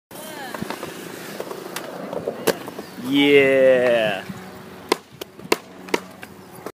Oscar Sosa - Grabbed Backslide 270 - Hansen Dam Skatepark
Here's another clip of Oscar Sosa AKA Maximilian Dabinport doing the damn thing.

This is banger drop Grabbed Backslide 270 done at the Hansen Dam Skatepark a couple weeks ago.

Thanks!